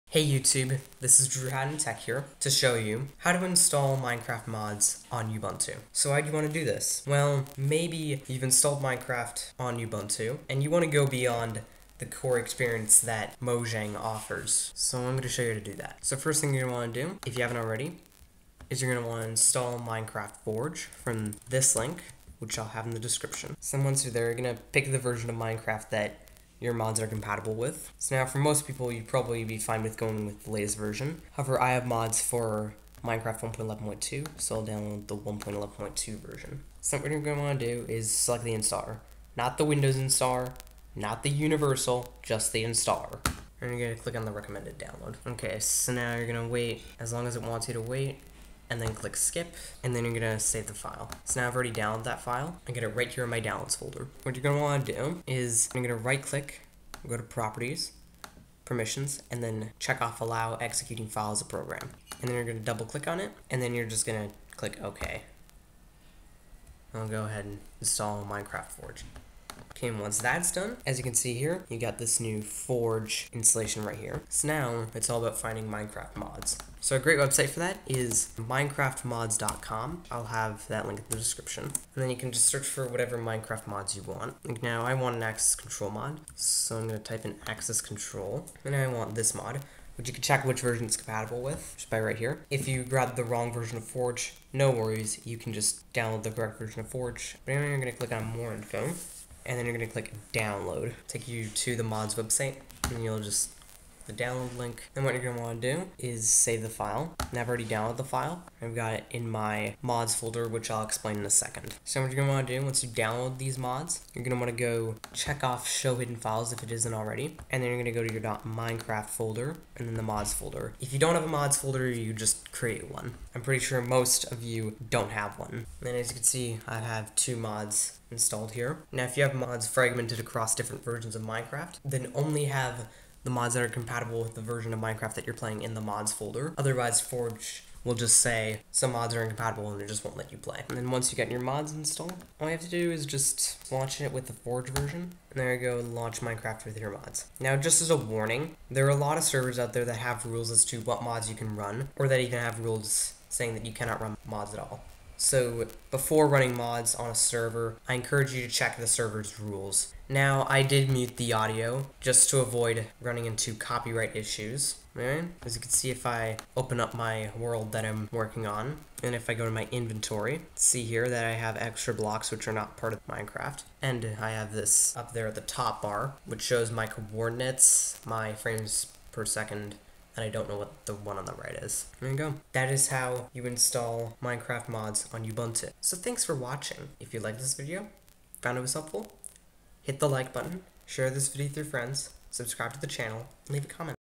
How to Install Minecraft Mods on Ubuntu
In this video, I show you how to install Minecraft mods on Ubuntu.

UPDATE: Before you can run the Minecraft Forge installer, you will need to right click on it, go to "Properties", "Open With", and make the OpenJDK Java Runtime the default application.

*If you already have Minecraft Forge installed, skip to 1:14.